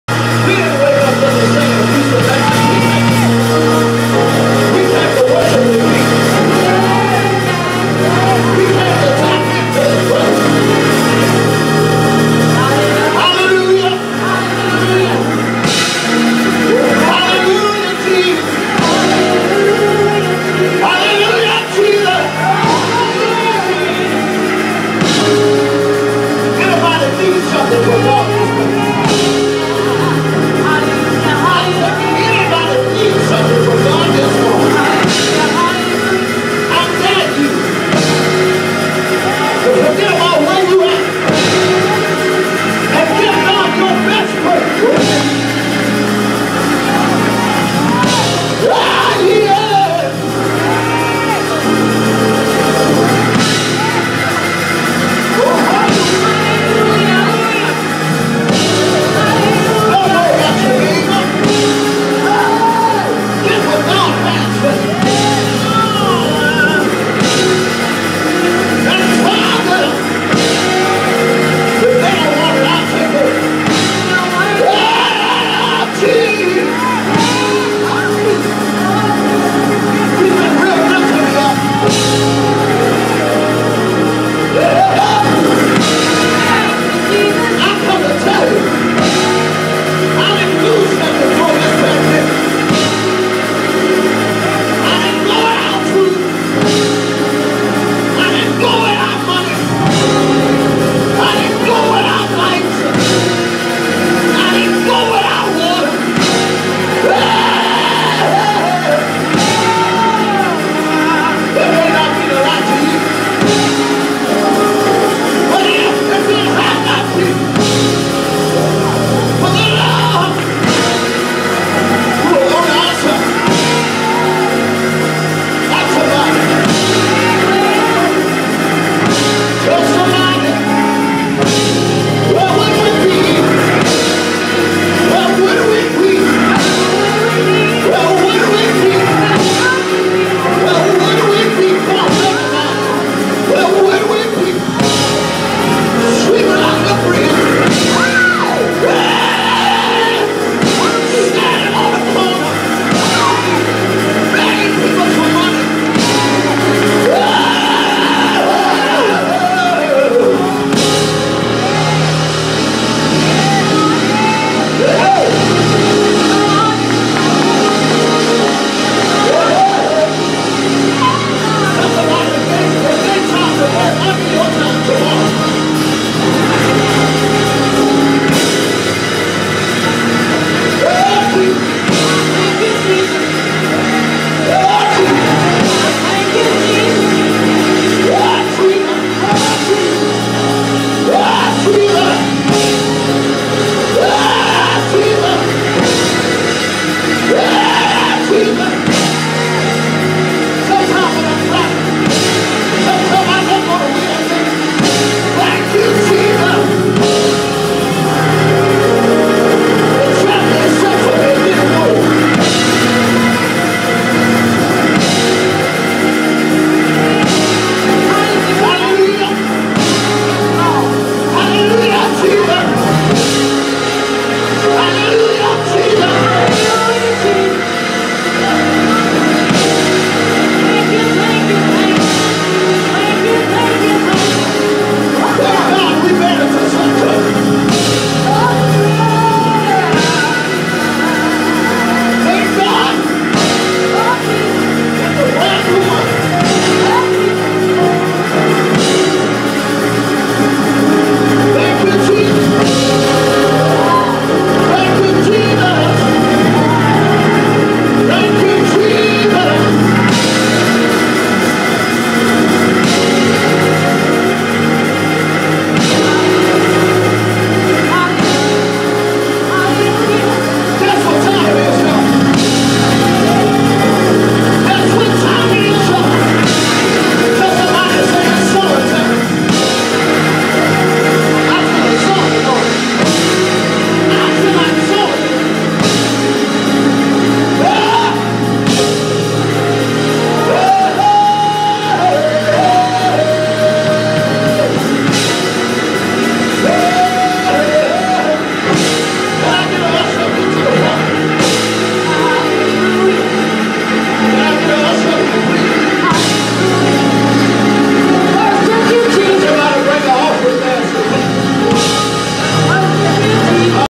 Praise Break II Minister Shaquille Davis
recorded at Greater Sixty Aid Baptist Church in Baton Rouge LA on 6 Sept 2020.